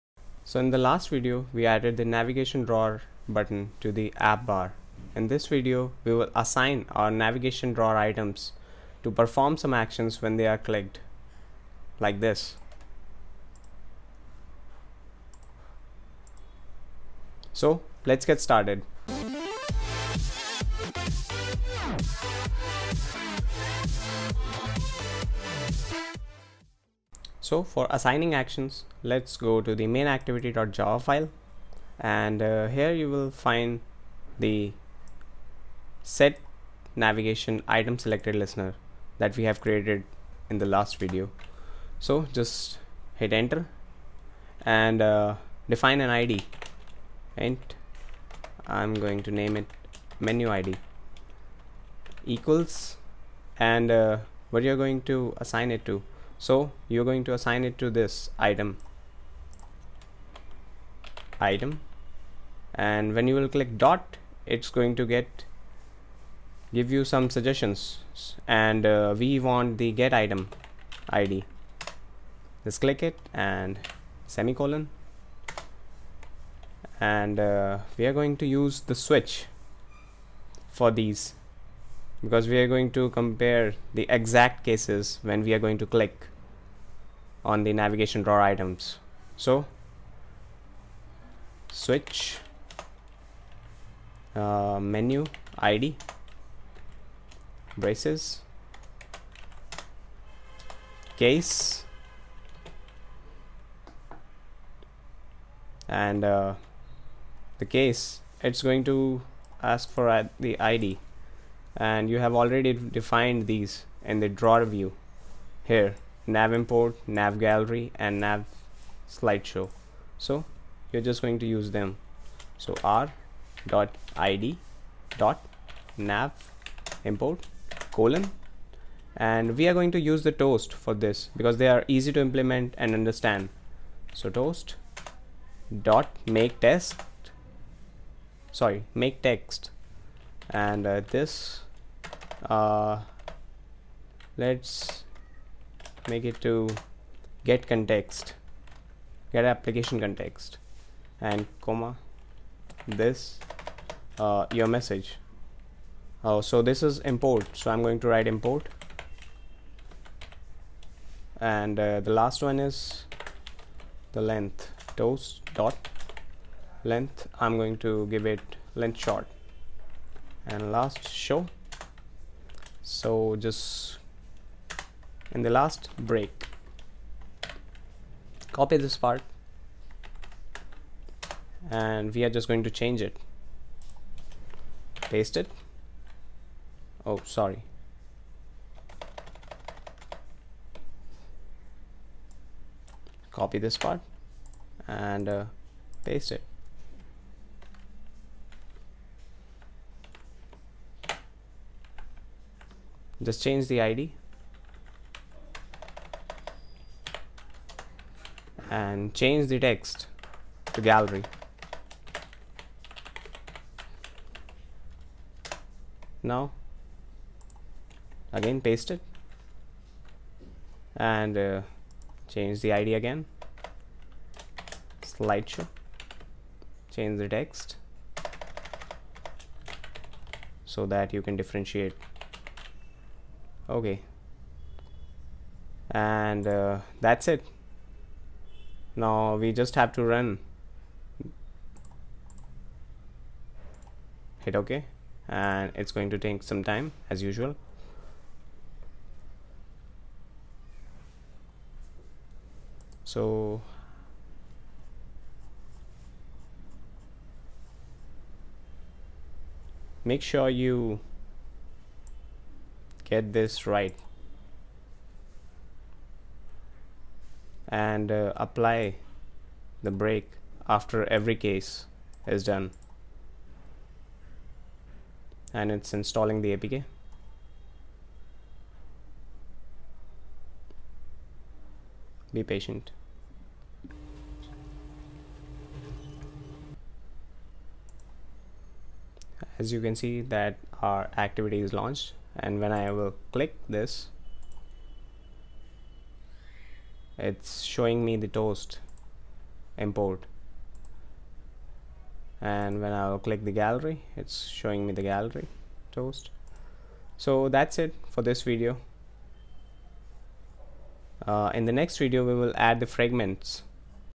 Android Navigation Drawer Using NavigationView Part 3 | Item Select | Android Studio Tutorial
If you have any request regarding Android, You can ask in the comments. Q & A's are welcome.